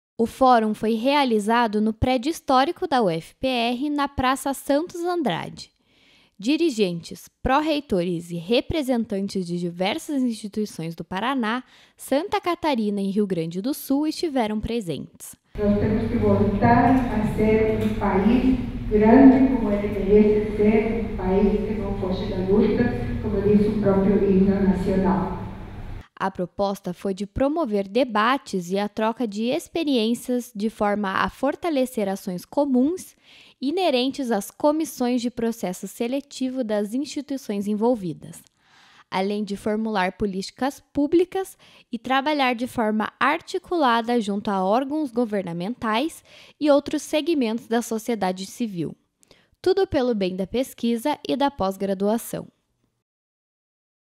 Informa | FOPROP SUL
A UFPR recebeu o Fórum de Pró-Reitores de Pesquisa e Pós-graduação das Universidades da Região Sul. O encontro aconteceu no Prédio Histórico e debateu atividades da área e políticas públicas para a pesquisa e pós-graduação.